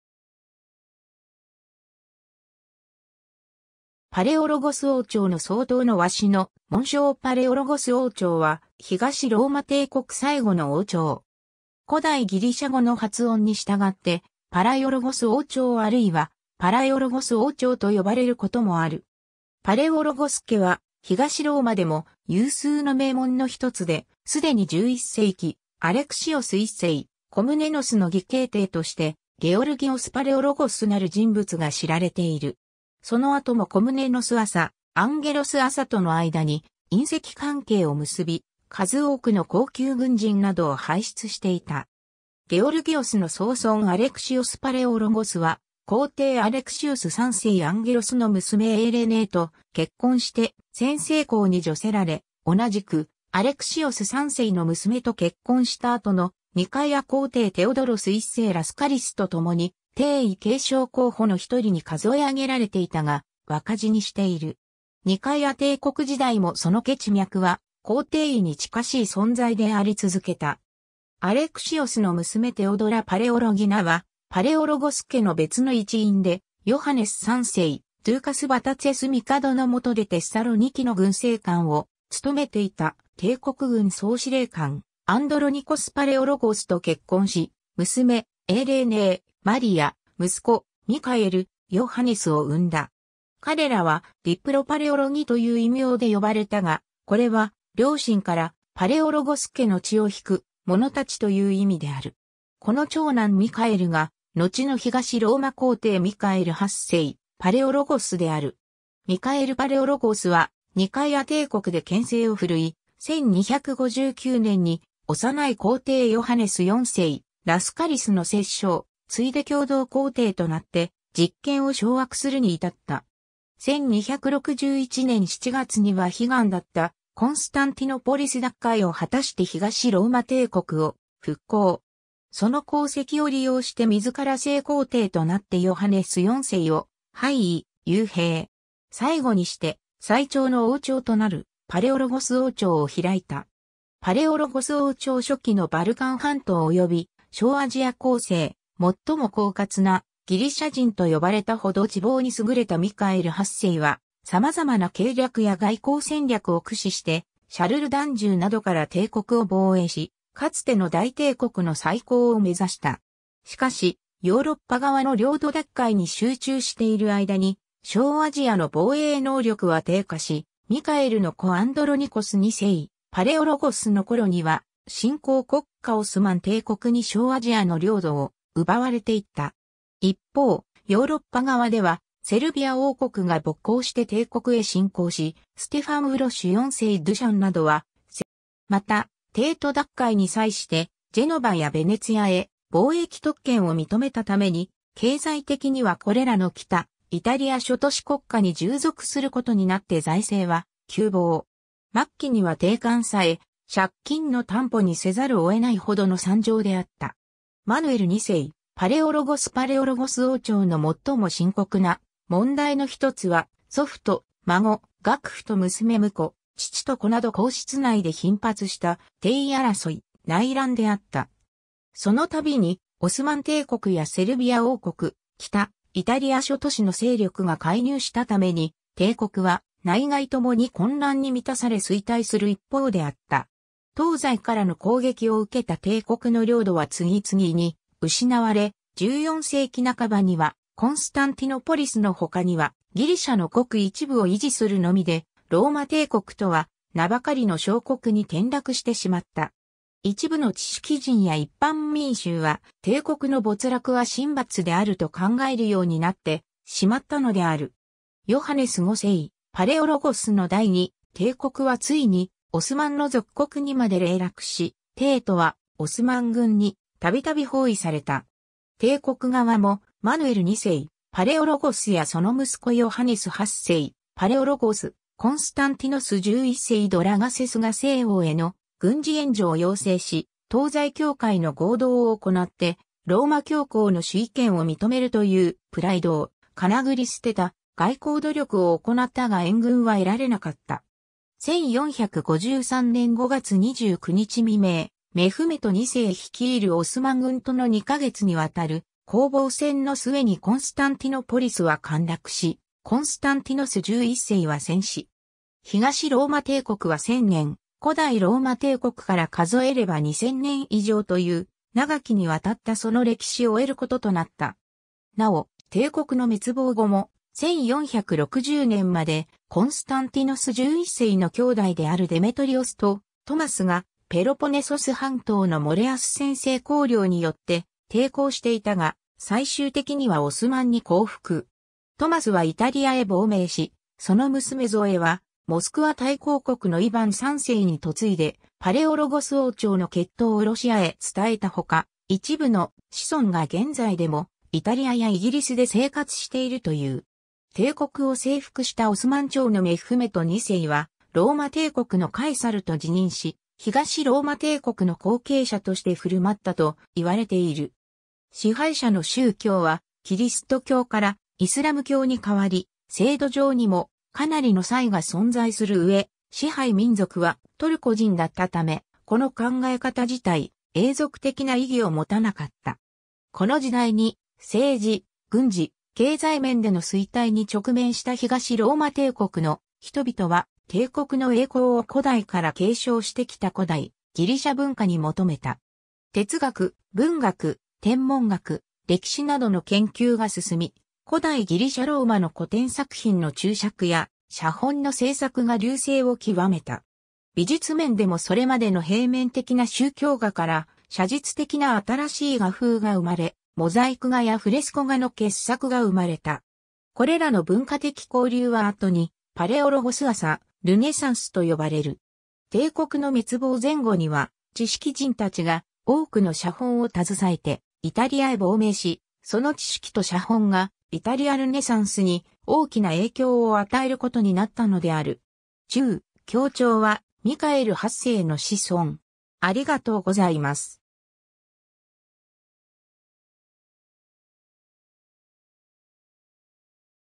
パレオロゴス王朝
パレオロゴス王朝の双頭の鷲の紋章 パレオロゴス王朝（パレオロゴスおうちょう、ギリシア語: Παλαιολόγος Palaiologos／女性形：Παλαιολογίνα Palaiologina パレオロギナ）は、東ローマ帝国最後の王朝（1261年 - 1453年）。
古代ギリシャ語の発音にしたがってパライオロゴス王朝あるいはパラエオロゴス王朝と呼ばれることもある。
 パレオロゴス家は東ローマでも有数の名門の一つで、既に11世紀、アレクシオス1世コムネノスの義兄弟としてゲオルギオス・パレオロゴスなる人物が知られている。
その後もコムネノス朝、アンゲロス朝との間に姻戚関係を結び、数多くの高級軍人などを輩出していた。
ゲオルギオスの曾孫アレクシオス・パレオロゴスは皇帝アレクシオス3世アンゲロスの娘エイレーネーと結婚して専制公に叙せられ、同じくアレクシオス3世の娘と結婚した後のニカイア皇帝テオドロス1世ラスカリスと共に帝位継承候補の一人に数え上げられていたが若死にしている。
 ニカイア帝国時代もその血脈は皇帝位に近しい存在であり続けた。
アレクシオスの娘テオドラ・パレオロギナはパレオロゴス家の別の一員で、ヨハネス3世ドゥーカス・ヴァタツェス帝の許でテッサロニキの軍政官を務めていた帝国軍総司令官（メガス・ドメスティコス）アンドロニコス・パレオロゴスと結婚し、娘エイレーネー、マリア、息子ミカエル、ヨハネスを産んだ。
彼らは「ディプロパレオロギ」という異名で呼ばれたが、これは両親からパレオロゴス家の血をひく者たちという意味である。
この長男ミカエルがのちの東ローマ皇帝ミカエル8世パレオロゴスである。
 ミカエル・パレオロゴスはニカイア帝国で権勢を揮い、1259年に幼い皇帝ヨハネス4世ラスカリスの摂政、ついで共同皇帝となって実権を掌握するに至った。
1261年7月には悲願だったコンスタンティノポリス奪回を果たして東ローマ帝国を復興。
その功績を利用して自ら正皇帝（ミカエル8世パレオロゴス）となってヨハネス4世を廃位・幽閉。
最後にして最長の王朝となるパレオロゴス王朝を開いた。
 パレオロゴス王朝初期（1265年）のバルカン半島及び小アジア 後世「最も狡猾なギリシャ人」と呼ばれたほど知謀に優れたミカエル8世は、さまざまな計略（第2リヨン公会議およびシチリアの晩鐘を参照）や外交戦略を駆使してシャルル・ダンジューなどから帝国を防衛し、かつての大帝国の再興を目指した。
しかし、ヨーロッパ側の領土奪回に集中している間に小アジアの防衛能力は低下し、ミカエルの子アンドロニコス2世パレオロゴスの頃には新興国家オスマン帝国に小アジアの領土を奪われていった。
一方、ヨーロッパ側ではセルビア王国が勃興して帝国へ侵攻し、ステファン・ウロシュ4世ドゥシャンなどは「セルビア人とローマ人の皇帝」を称してコンスタンティノポリス奪取を標榜するまでになった。
また、帝都奪回に際してジェノヴァやヴェネツィアへ貿易特権を認めたために、経済的にはこれらの北イタリア諸都市国家に従属することになって財政は窮乏。
末期には帝冠さえ借金の担保にせざるを得ないほどの惨状であった。
 マヌエル2世パレオロゴス パレオロゴス王朝の最も深刻な問題の一つは、祖父と孫、岳父と娘婿、父と子など皇室内で頻発した帝位争い、内乱であった。
そのたびにオスマン帝国やセルビア王国、北イタリア諸都市の勢力が介入したために、帝国は内外共に混乱に満たされ衰退する一方であった。
東西からの攻撃を受けた帝国の領土は次々に失われ、14世紀半ばにはコンスタンティノポリスの他にはギリシャのごく一部を維持するのみで、「ローマ帝国」とは名ばかりの小国に転落してしまった。
一部の知識人や一般民衆は、帝国の没落は神罰であると考えるようになってしまったのである。
 ヨハネス5世パレオロゴスの代に、帝国はついにオスマンの属国にまで零落し、帝都はオスマン軍にたびたび包囲された。
帝国側もマヌエル2世パレオロゴスやその息子ヨハネス8世パレオロゴス・コンスタンティノス11世ドラガセスが西欧への軍事援助を要請し、東西教会の合同を行ってローマ教皇の首位権を認めるというプライドをかなぐり捨てた外交努力を行ったが援軍は得られなかった。
 1453年5月29日未明、メフメト2世率いるオスマン軍との2ヶ月にわたる攻防戦の末にコンスタンティノポリスは陥落し（コンスタンティノープルの陥落）、コンスタンティノス11世は戦死。
東ローマ帝国は1000年、古代ローマ...